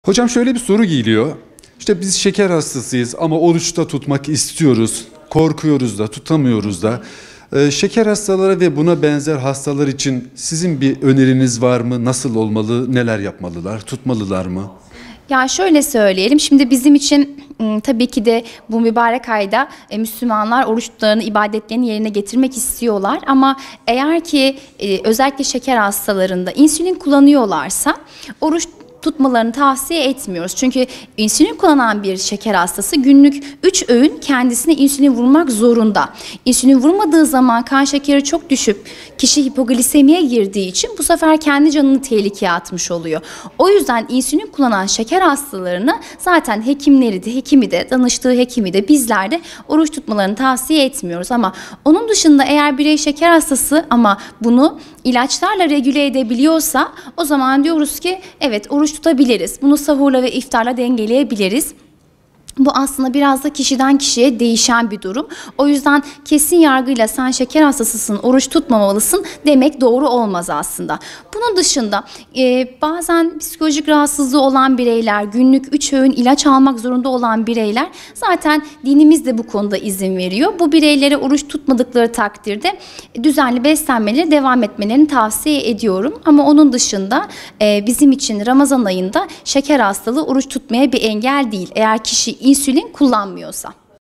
Şeker Hastaları Oruç Tutmalı Mı?
Uzman Diyetisyen Nagihan Özçelik yanıtladı: Şeker hastaları oruç tutmalı mı?